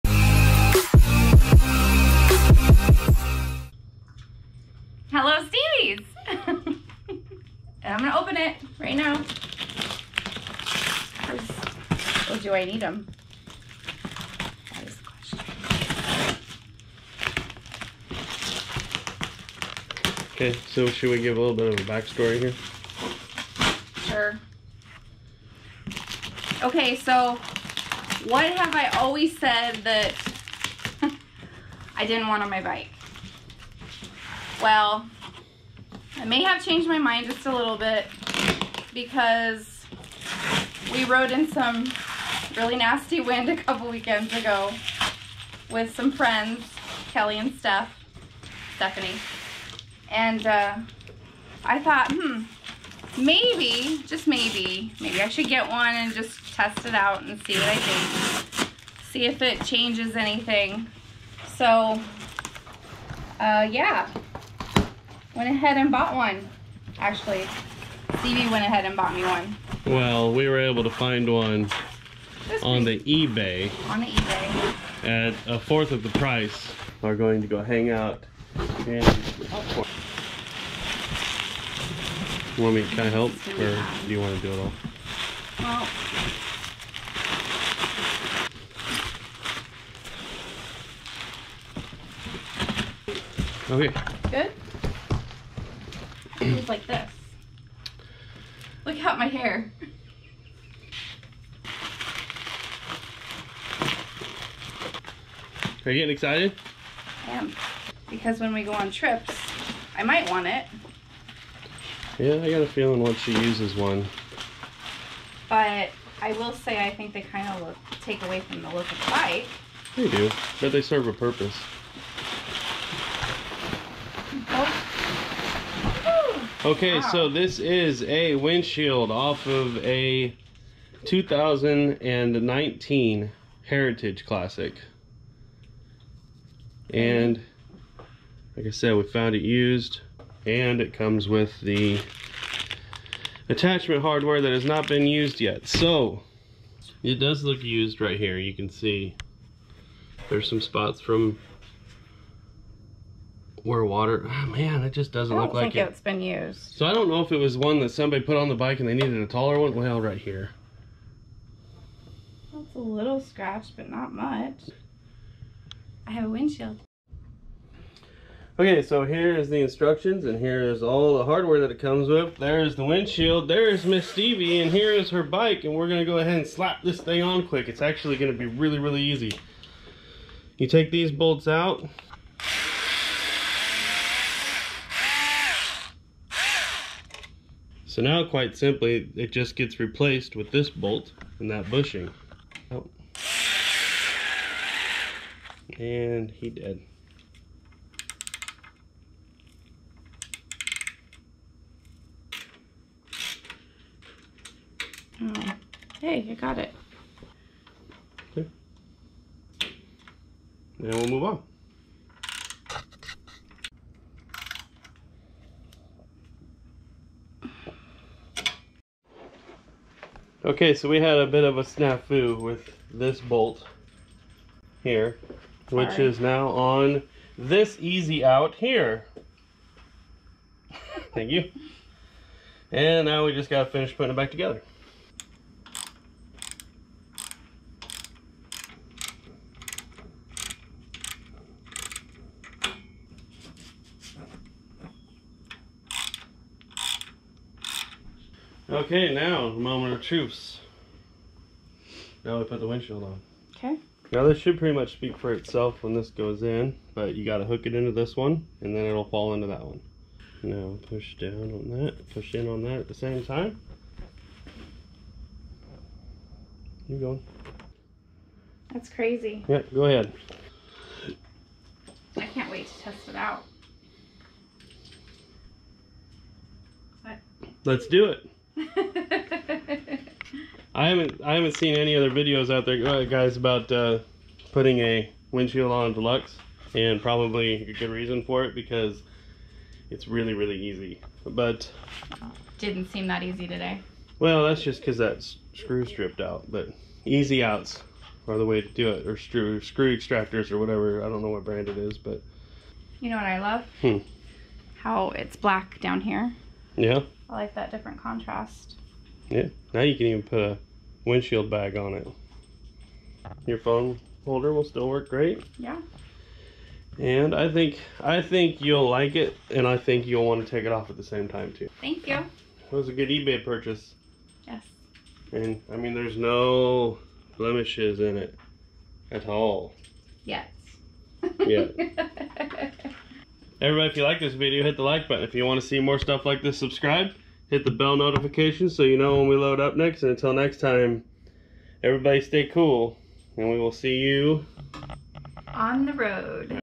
2020 Softail Deluxe Windshield Install
Installing a 2019 Heritage Windshield on a 2020 Softail Deluxe.

Mailing Address
Stevy Faux
P.O.Box 261